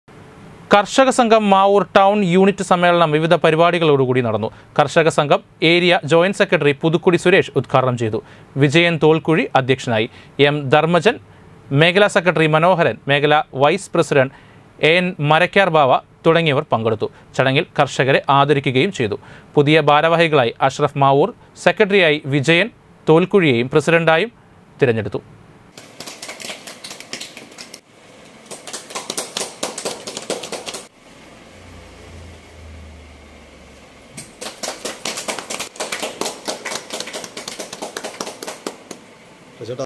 കർഷക സംഘം മാവൂർ ടൗൺ യൂണിറ്റ് സമ്മേളനം വിവിധ പരിപാടികളോടെ നടന്നു
കർഷക സംഘം മാവൂർ ടൗൺ യൂണിറ്റ് സമ്മേളനം വിവിധ പരിപാടികളോടെ നടന്നു. കർഷക സംഘം ഏരിയ ജോ. സെക്രട്ടറി പുതുക്കുടി സുരേഷ്   ഉദ്ഘാടനം ചെയ്തു.വിജയൻ തോൾകുഴി അധ്യക്ഷനായി